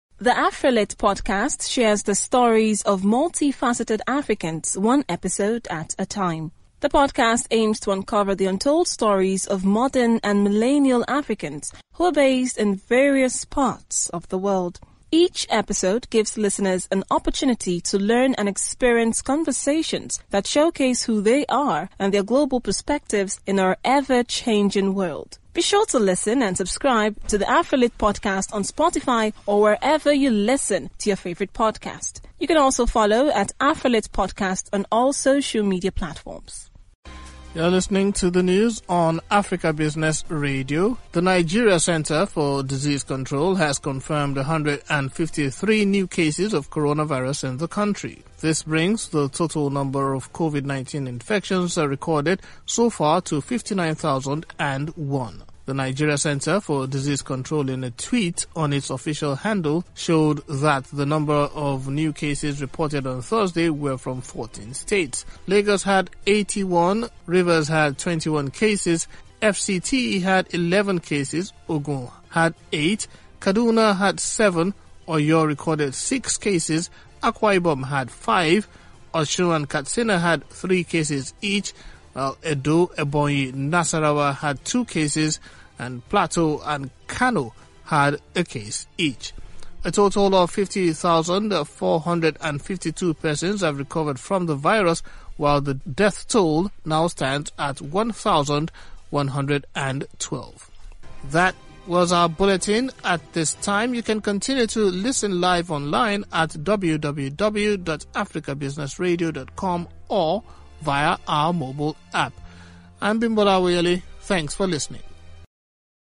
Nigeria Records 153 New COVID-19 Cases
The Nigeria Centre for Disease Control has confirmed 153 new cases of coronavirus in the country. This brings the total number of COVID-19 infections recorded in the country so far to 59,001.
The Nigeria Centre for Disease Control (NCDC) in a tweet on its official handle, showed that the new cases reported on Thursday were from 14 states.
Lagos had Eighty-one case, Rivers had twenty-one cases, (21), FCT had eleven cases, Ogun had eight case, Kaduna had seven case, Oyo had six case, Akwa Ibom had five case, Osun and Katsina had three cases each, Edo, Ebonyi Nasarawa had two cases while Plateau and Kano had one case each.
A total of 50,452 persons have recovered from the virus while the death toll now stands at 1,112.

· Afrolit Podcast: Hosted by Ekua PM, Afrolit shares the stories of multi-faceted Africans one episode at a time.